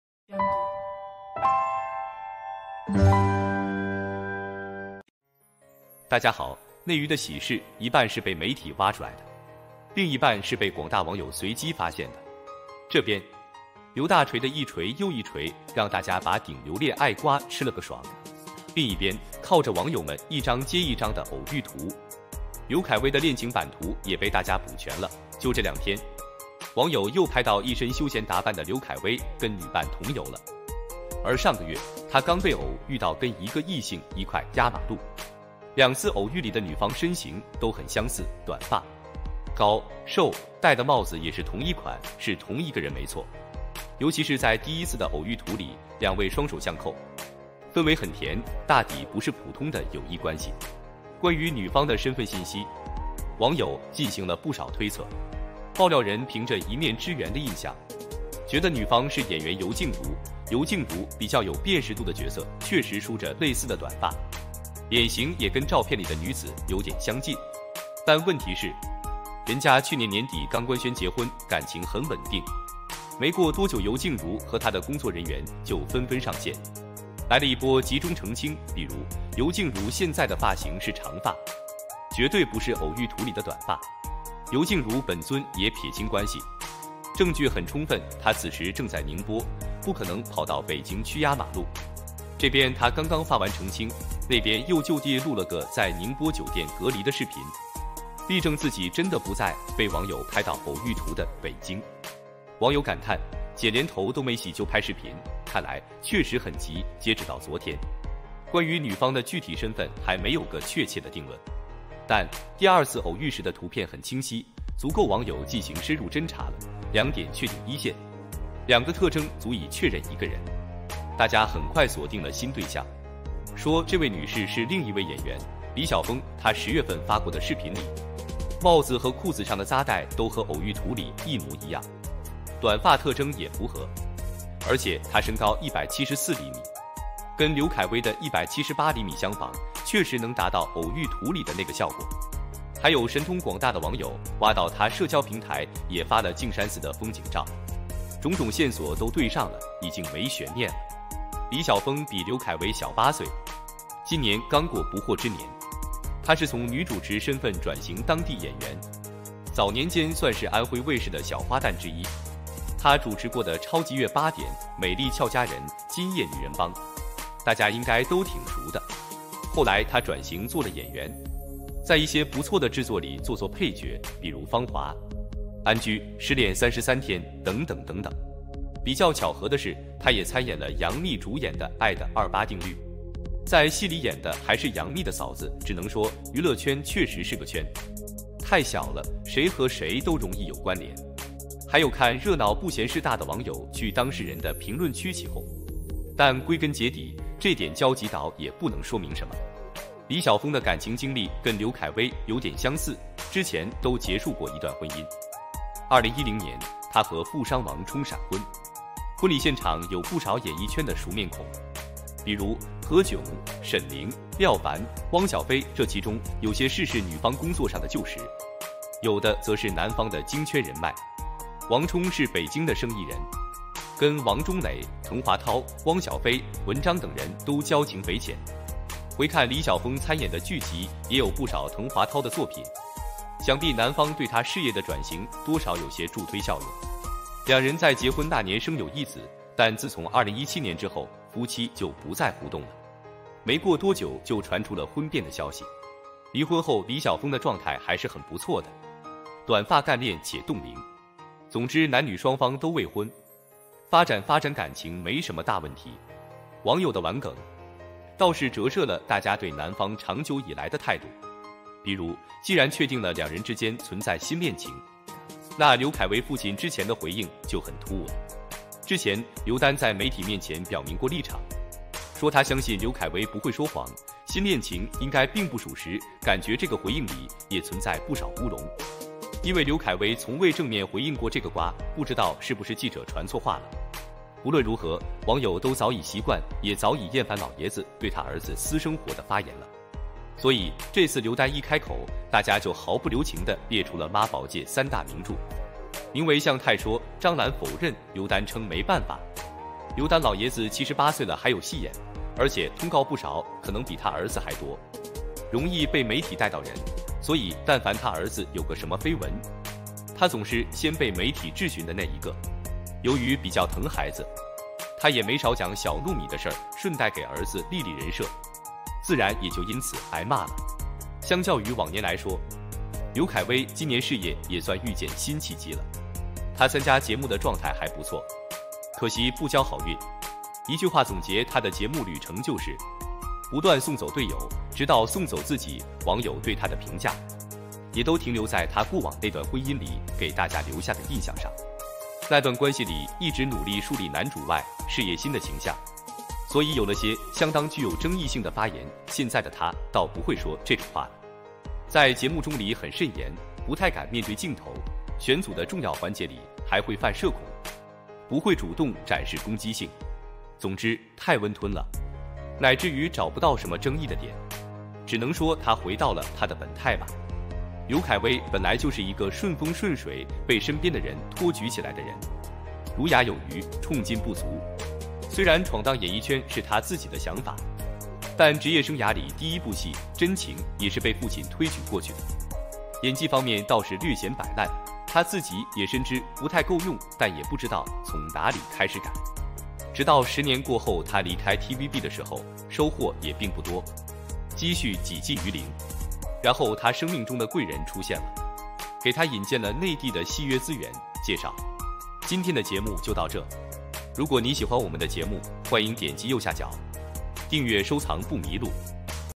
離婚多年，終于迎來第二春？
這邊，劉大錘的一錘又一錘讓大家把頂流戀愛瓜吃了個爽;兩次偶遇里的女方身形都很相似，戴的帽子也是同一款，是同一個人沒錯。